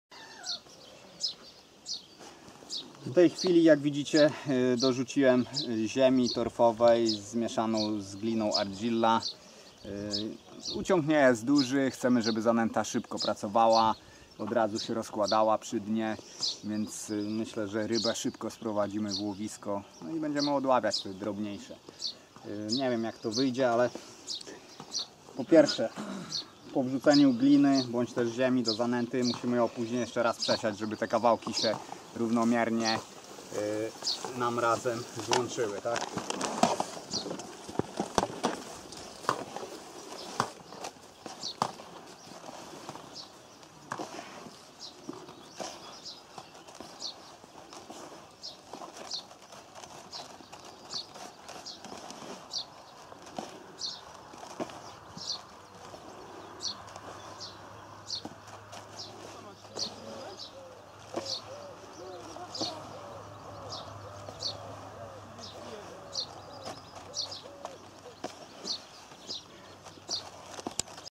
TeamShark Piotr Siółkowski - przygotowanie zanęty IV
Mieszanie zanęty z gliną